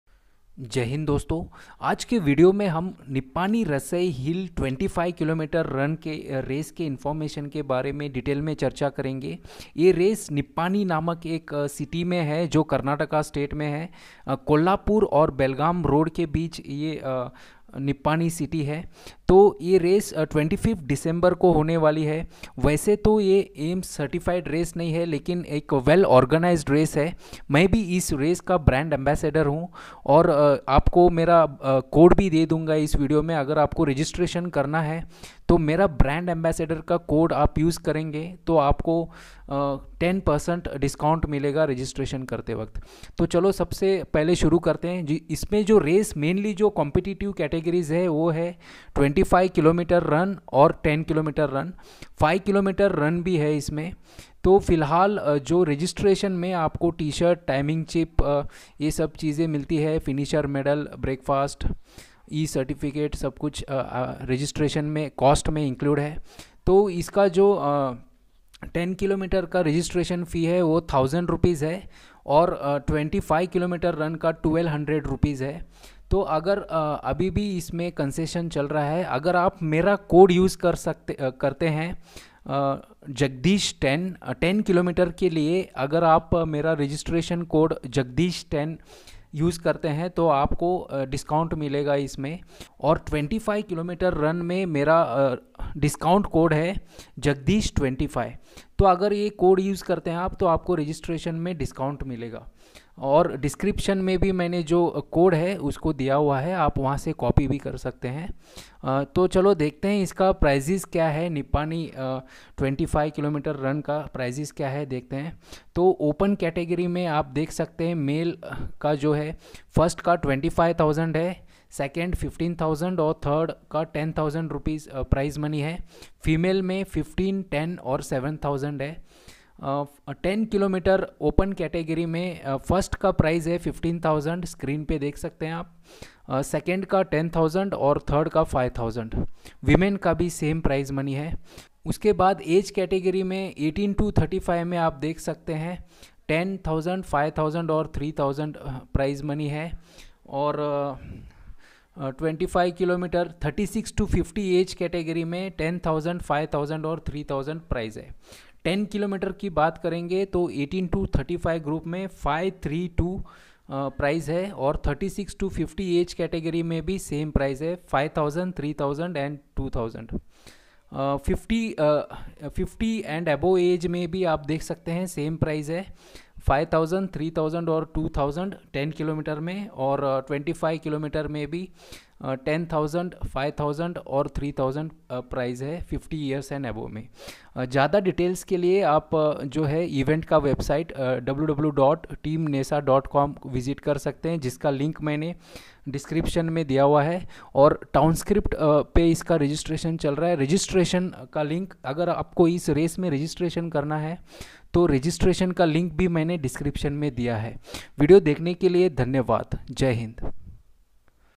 Nippani Rasai Hill 25Km Run | Race Information, Registration and Prize Money details
Registration link for Nippani Rasai Hill Marathon -